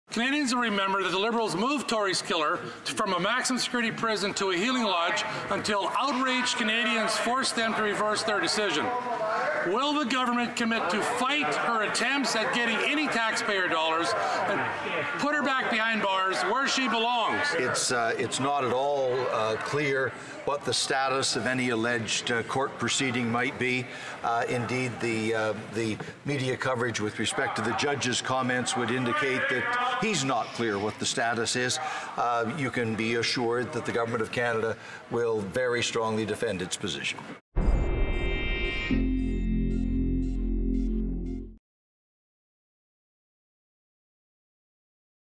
Goodale says feds will defend against any legal action from Tori Stafford killer
Public Safety Minister Ralph Goodale says the federal government will defend itself against any legal action from Terri-Lynne McClintic, who was sentenced to life in prison in 2010 for her role in the first-degree murder of eight-year-old Tori Stafford. During question period Tuesday, Conservative MP Glen Motz asked about McClintic, whose transfer to an Indigenous healing lodge was reversed following a public outcry last year. (May 14, 2019.)